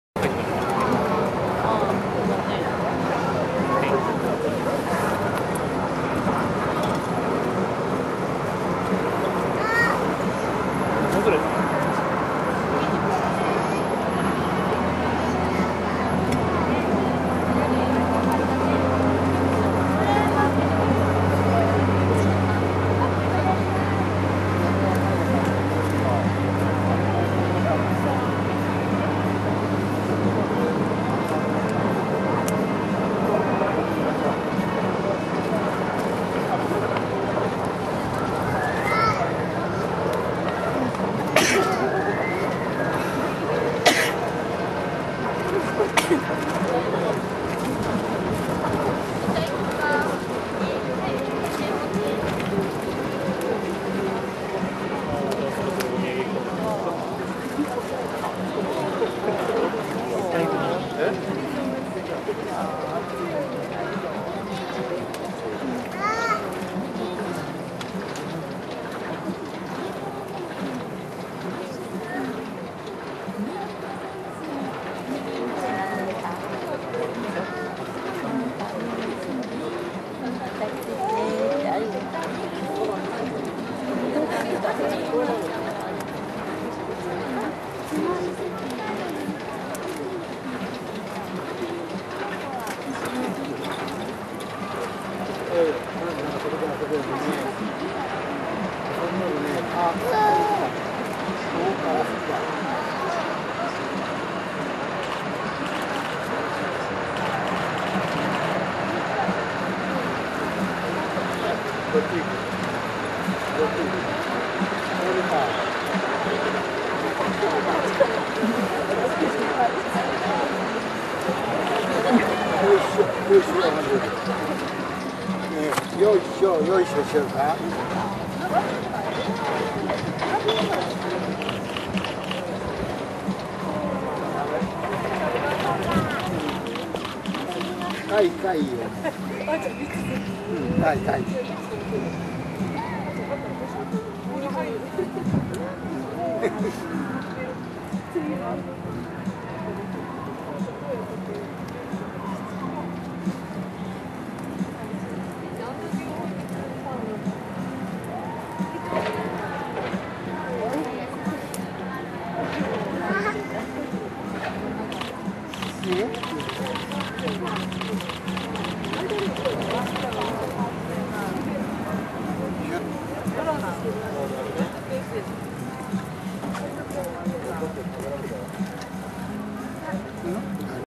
Meihin and Eihin of 2012 May at Wakayama, Japan #4
Meihin(female) and Eihin(male) are twins.  They are 3 years and 8-months old then.  They are born at Wakayama, Japan.